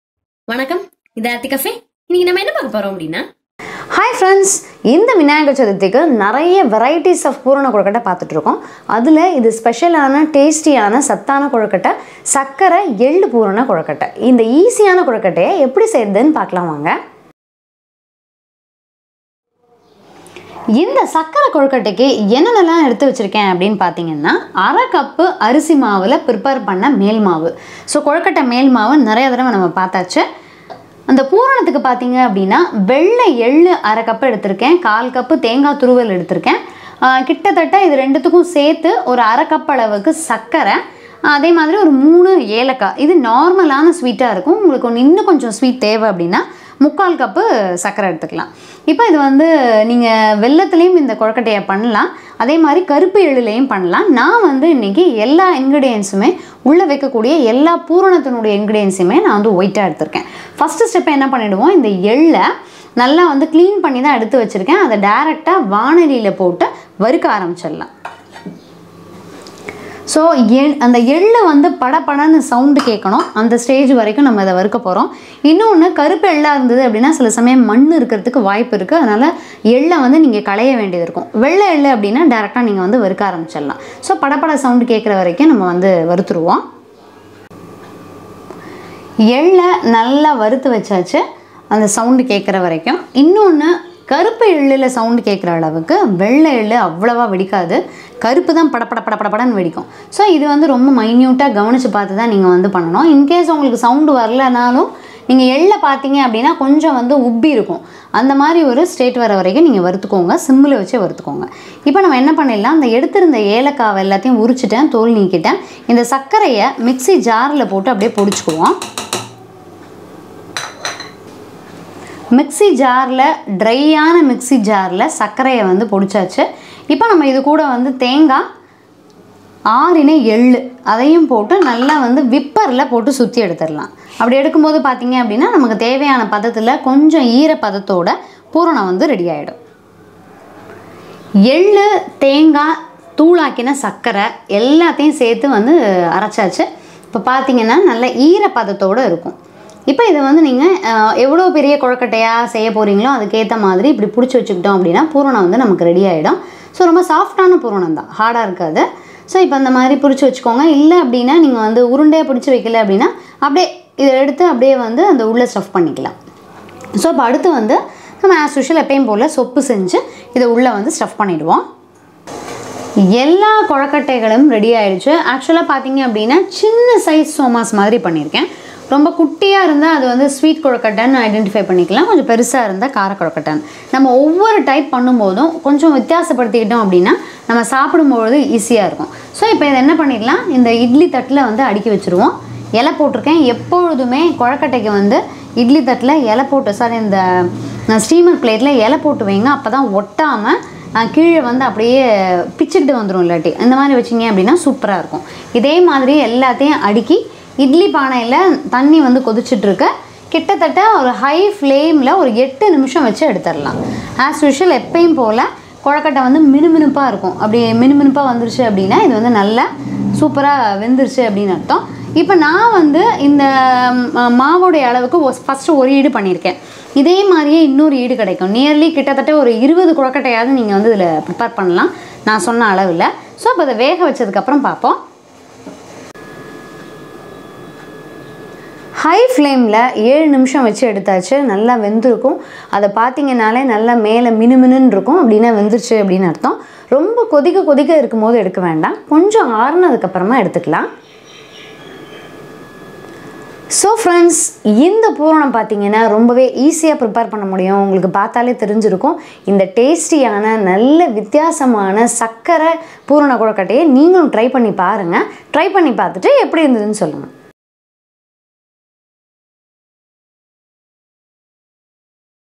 எள்ளு பூரண இனிப்பு கொழுக்கட்டை Ellu Poorna Kozhukattai | Vinayagar Chaturthi 2021
தேவையானப் பொருட்கள்

கொழுக்கட்டை மேல் மாவு - 1/2 கப் அரிசிமாவில் செய்தது ( செய்முறை Link கீழே கொடுக்கப்பட்டுள்ளது)
வெள்ளை எள்ளு - 1/2 கப்
தேங்காய் துருவல் - 1/4 கப்
சர்க்கரை - 1/2 கப்
ஏலக்காய் - 3